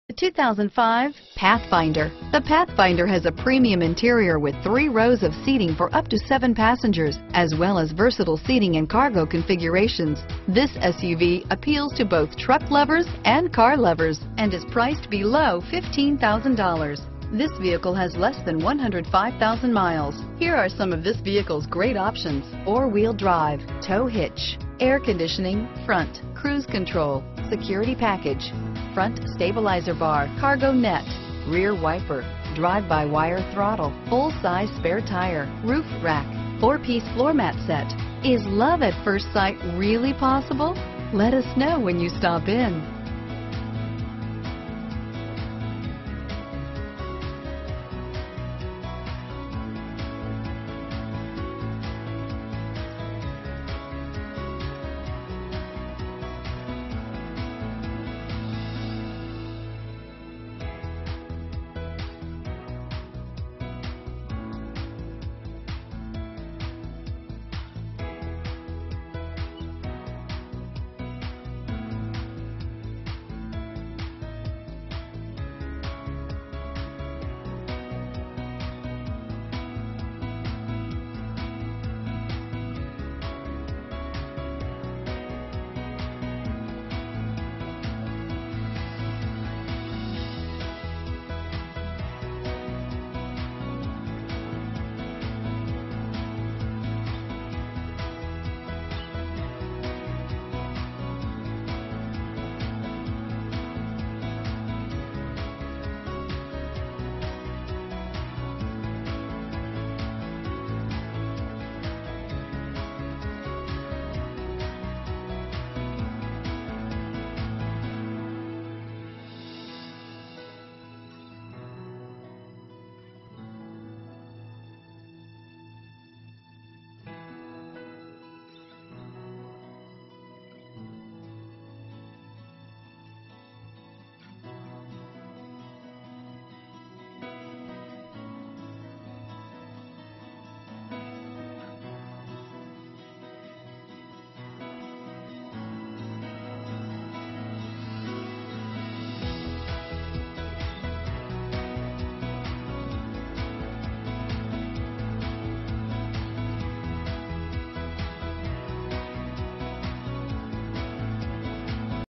2005 Nissan Pathfinder Tulsa, Broken Arrow, Owasso, Bixby, Green Country, OK G3380A
2005 Nissan Pathfinder  - Stock#: G3380A - VIN#: 5N1AR18W15C789673

Ferguson Superstore
1601 N Elm Place

4WD and CLEAN CARFAX ONE OWNER. This is your chance to be the second owner of this charming-looking 2005 Nissan Pathfinder, kept in great condition by its original owner. Some manufacturers cut corners to save money, but Nissan didn't try to shave off a single penny when building this excellent Pathfinder. We've Got What You're Looking For!